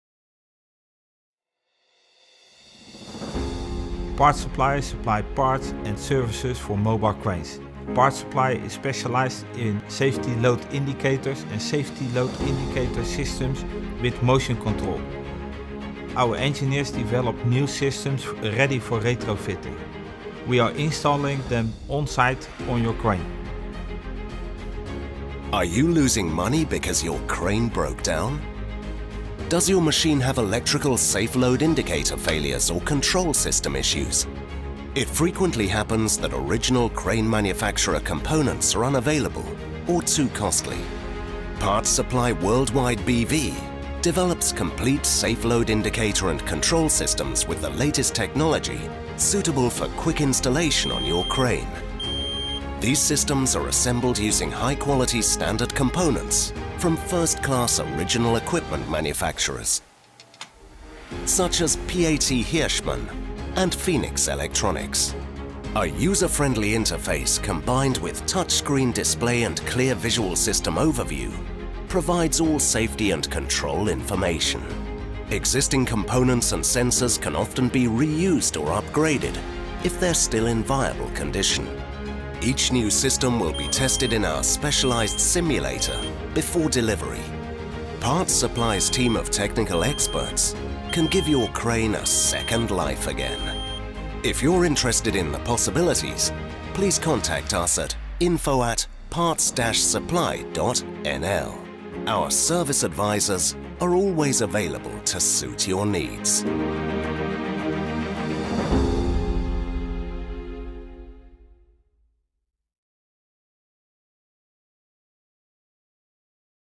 MOTION CONTROL SYSTEM FOR ALL CRANES | Parts Supply Worldwide BV
MOTION CONTROL SYSTEM FOR (MOBILE) CRANES

Does your machine have motion control failures.

Hirschmann Motion control and Safe Load Indicator systems ensures the safety, prevent accidents and reduce downtime during lifting operations.

These systems are assembled using  the high quality standard components from first class OEM such as PAT Hirschmann and Phoenix electronics.
User friendly handling and logical menu navigation as well as contemporary display visualization comply with the present requirements of automation on construction sites.

Features
- Often the system doesn't need to be calibrated!
- The system is made for easy trouble shooting
- Thanks to virtual wall function operator is able now to work in limited spaces
- If you choose, the cable reel as well the A2B switch or other sensors can be reused, thereby saving on the costs for purchasing of the new sensors
- And in order to safe installation time on-site and space this -
 - Hirschmann system is connected with can-bus connectors.
- Existing components and sensors can be re-used or upgraded